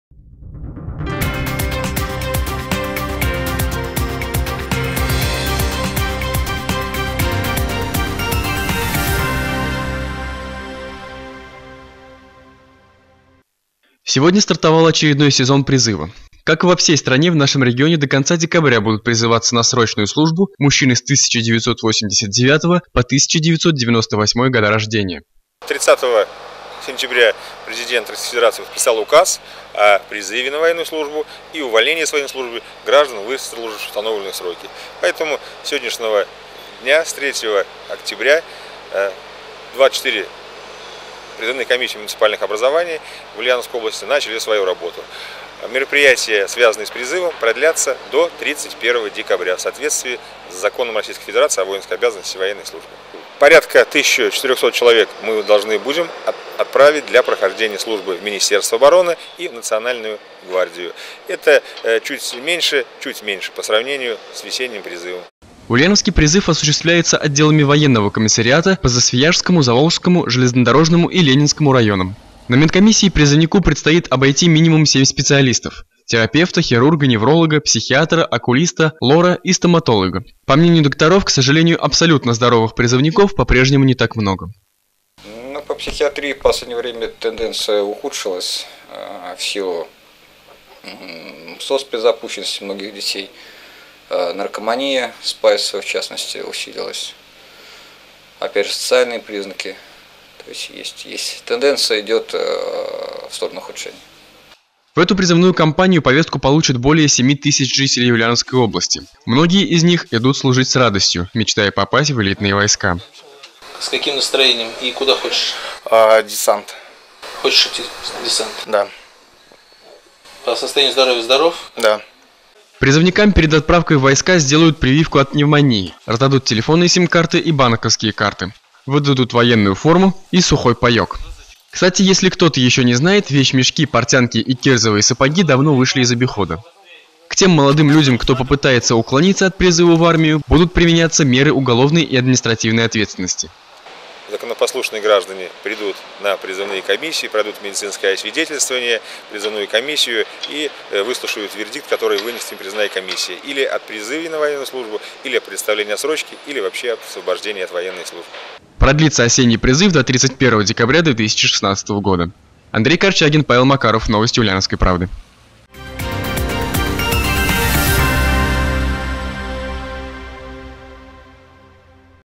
В Ульяновской области начался осенний призыв
Сегодня стартовал очередной сезон призыва. Как и во всей стране, в нашем регионе до конца декабря будут призываться на срочную службу мужчины с 1989 по 1998 год рождения.